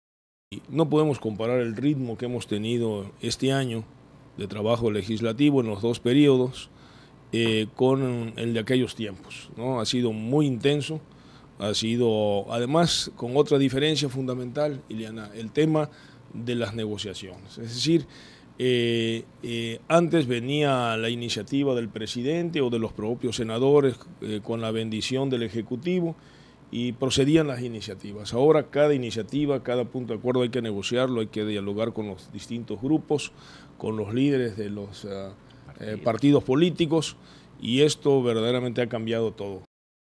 Humberto Mayans hablama de las diferencias en las negociaciones
En entrevista en El Informativo Primera Edición a Humberto Mayans, Senador de la República.